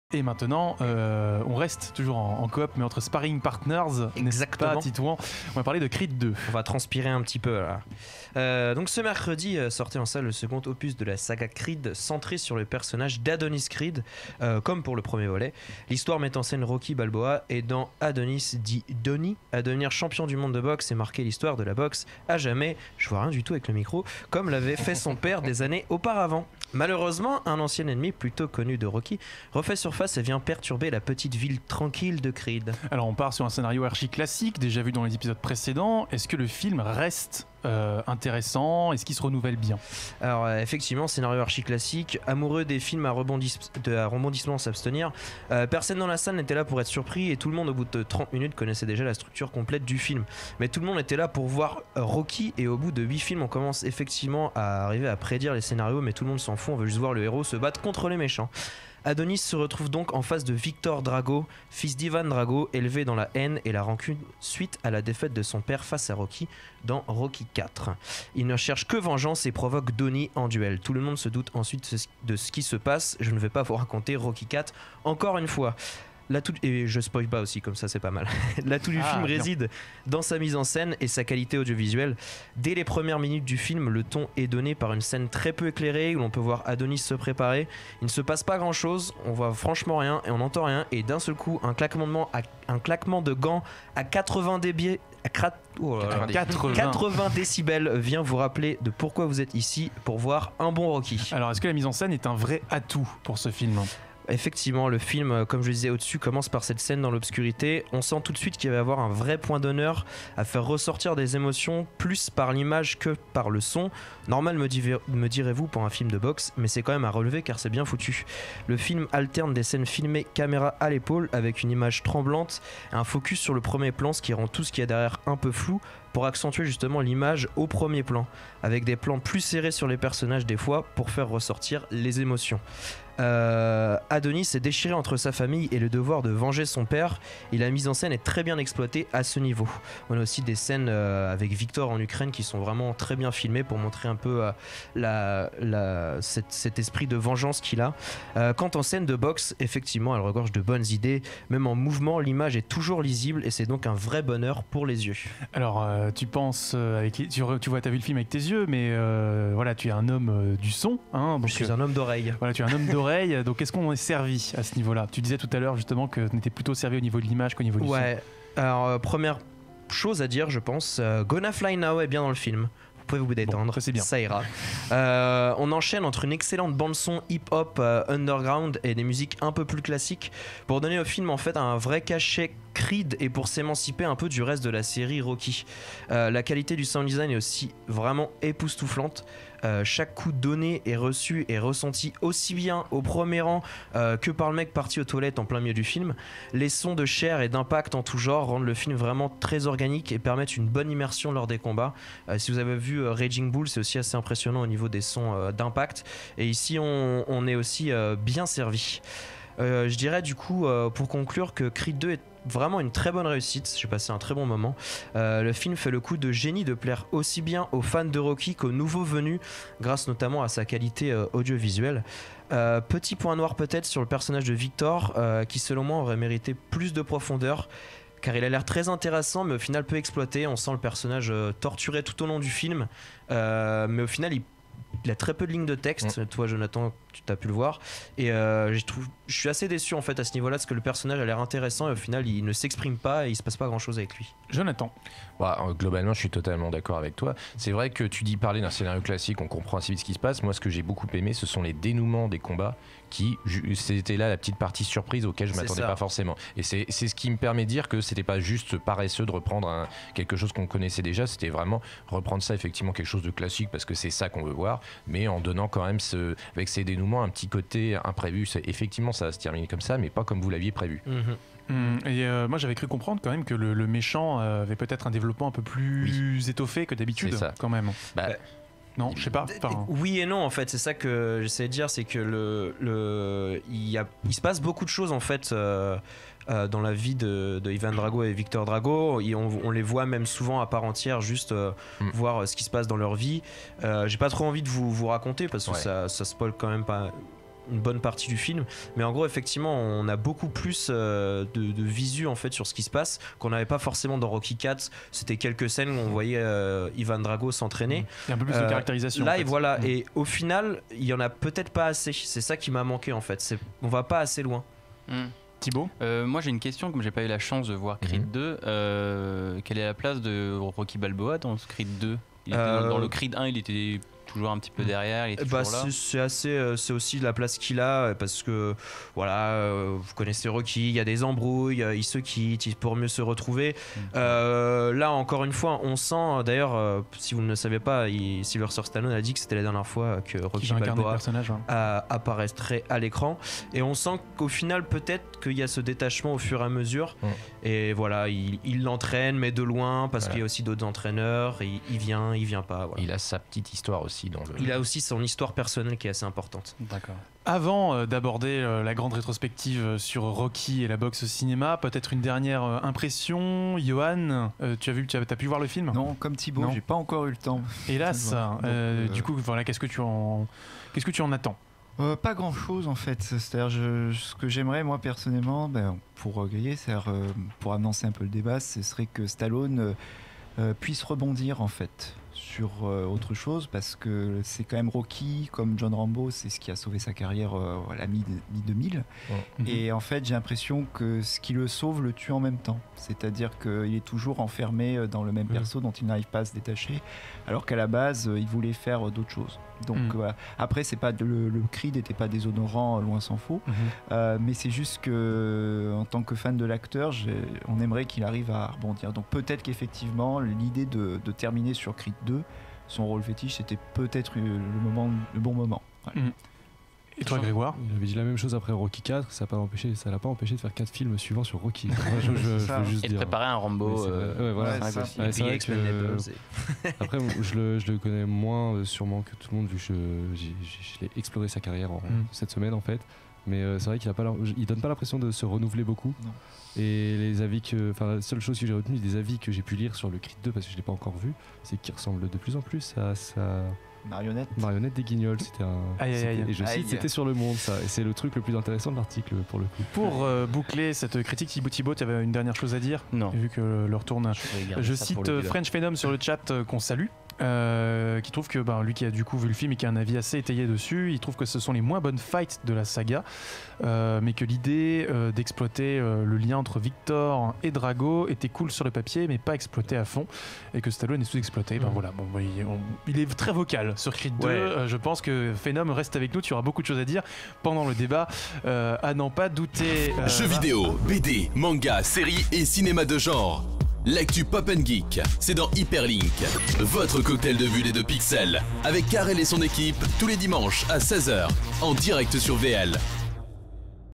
HyperLink #79 : Critique - Creed II w/ Yoan ORSZULIK & Thibaud GOMES-LEAL
Après avoir reconquis son nom dans le premier opus signé Ryan Coogler, Adonis Creed va devoir jouer des coudes avec Victor Drago, fils du redoutable boxeur russe qui a tué son père (mais si, souvenez-vous, dans Rocky IV…). Pour ce match retour aux airs de revanche, le réalisateur du premier opus laisse sa place à Steven Caple Jr. Passation réussie ? Titouan rend son verdict.